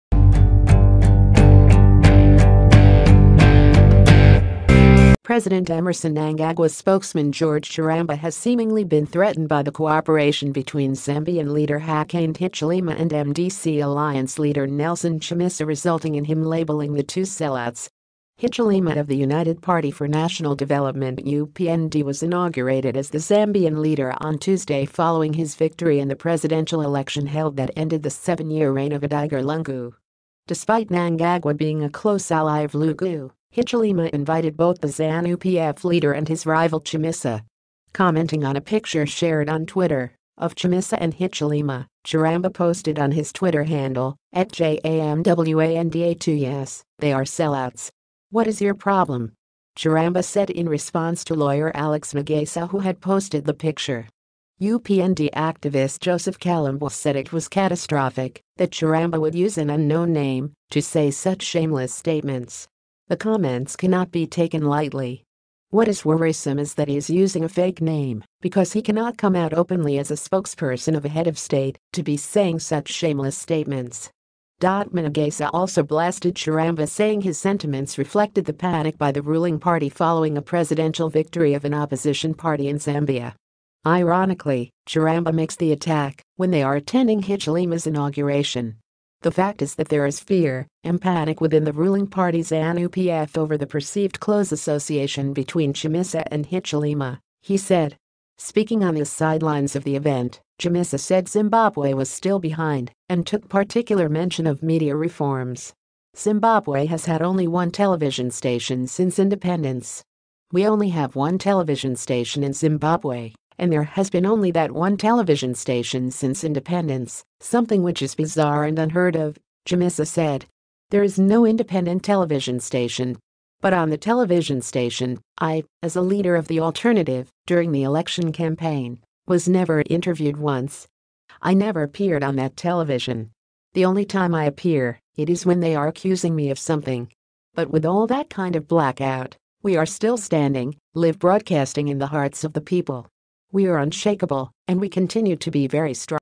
Mnangagwa spokesman says Zambia’s Hichilema and Chamisa ‘are sellouts’
President Emmerson Mnangagwa’s spokesman George Charamba has seemingly been threatened by the cooperation between Zambian leader Hakainde Hichilema and MDC Alliance leader Nelson Chamisa resulting in him labelling the two “sellouts”.